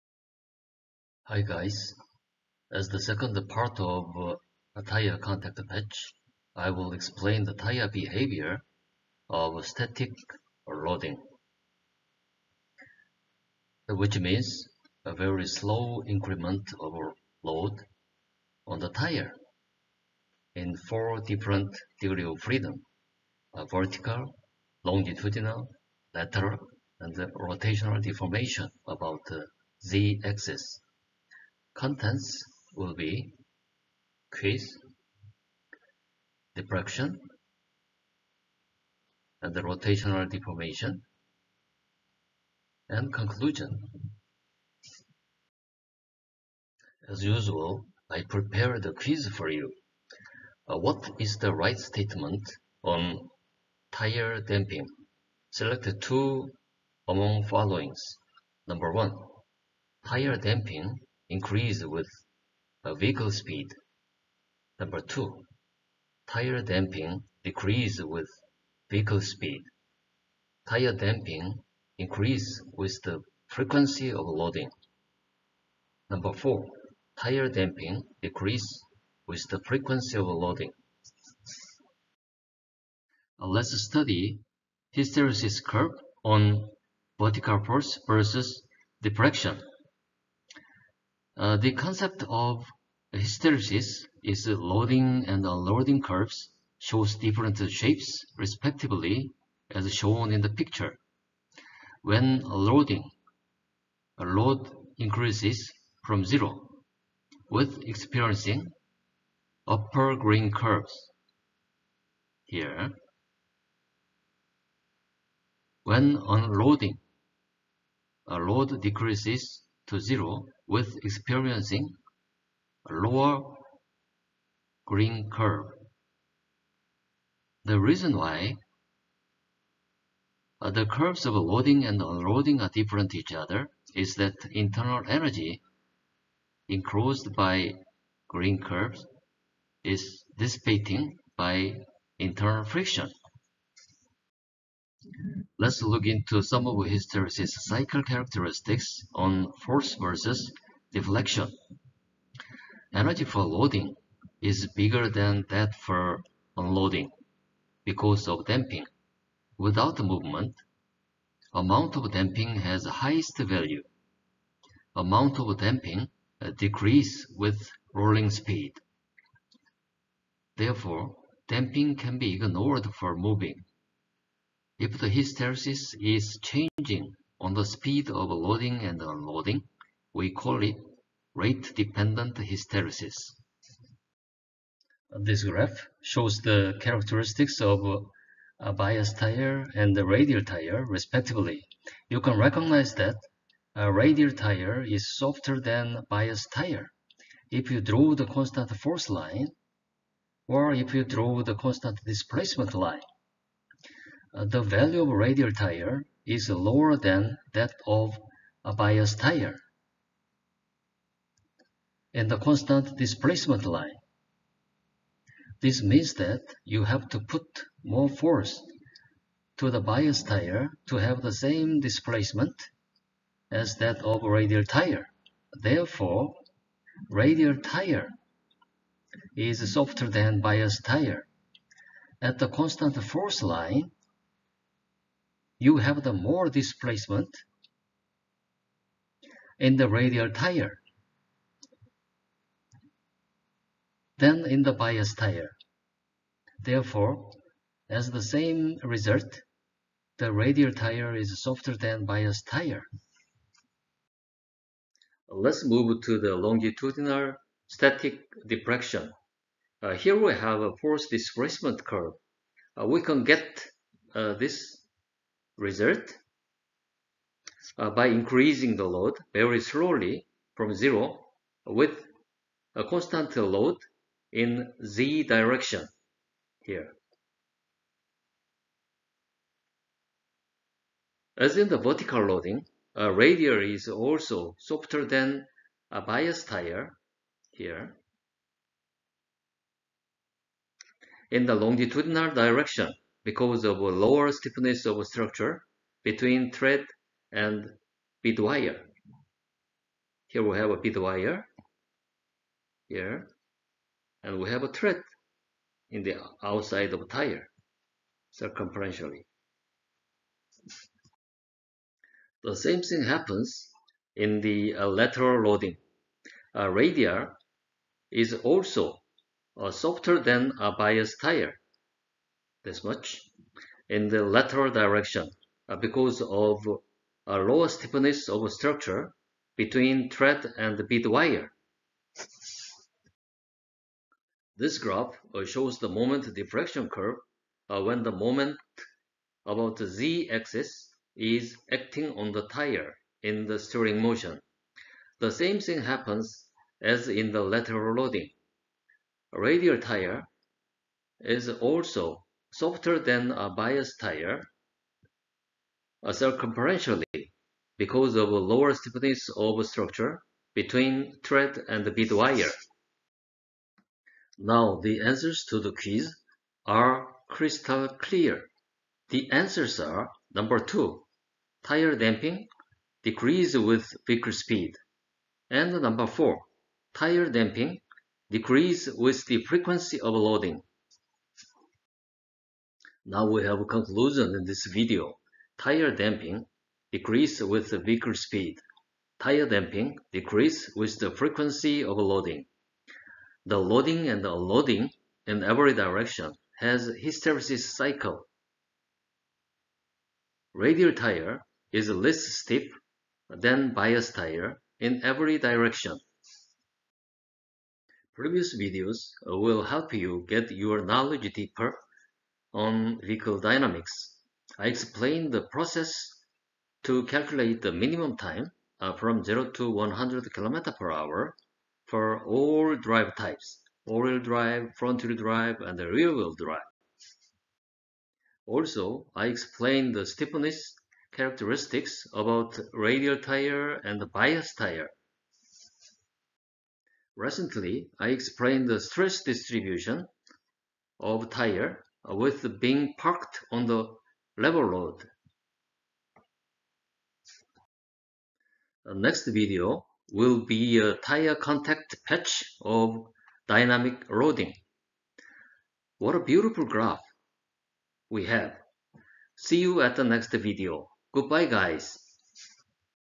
E0013 tire contact patch(static 2), Tire Stiffness - all DOF - EulSeoggy Ko
This video explains vertical, lateral, longitudinal, rotational stiffness. Tire stiffness gets different depending on tire types. Radial Tire is stiffer than bias tire in all direction because radial tire has hard belts circumferentially. Tires deform continuously between tire contact patch and other area. In this process, the deformation energy is not fully recovered when tire's bouncing back. The remaining energy not for bouncing back is disappeared in the air in the form of thermal energy. This is the internal energy and we call it hysteresis damping. Therefore, the graph of force versus displacement when increasing the load does not coincide with the graph of that when decreasing the load. the line for increasing is always higher than that for decreasing. The area enclosed by those two lines represents the thermal energy disappeared in the air. This energy reduces when the vehicle speed increasing.